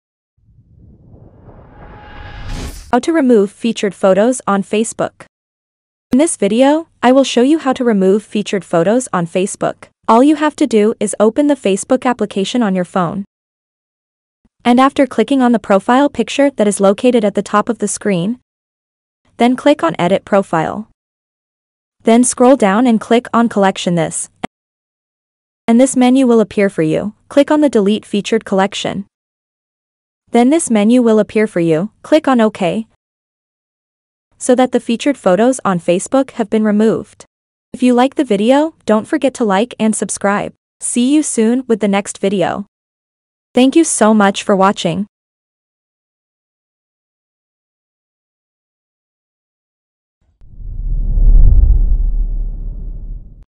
How To Remove Featured Photos On Facebook (EASY)
In this video, I will show you How To Remove Featured Photos On Facebook (EASY)